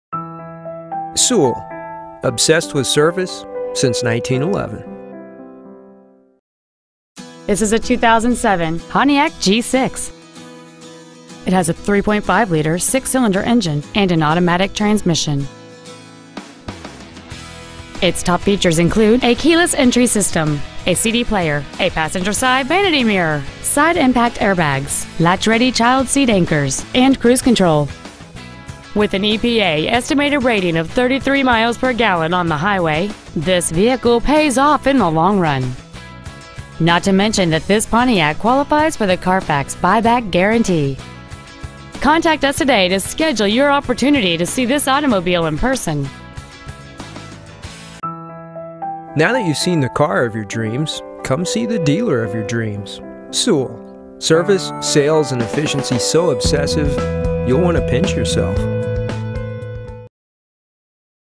2007 Pontiac G6 Dallas TX
Year : 2007

 Make : Pontiac

 Model : G6

 Engine : 3.5L V6 SFI

 Trans . : Automatic

 Exterior : White

 Miles : 98,207

 Interior : Black

 6211 Lemmon Ave

 Mission

 Values

 Pre-Owned 2007 Pontiac G6 Dallas TX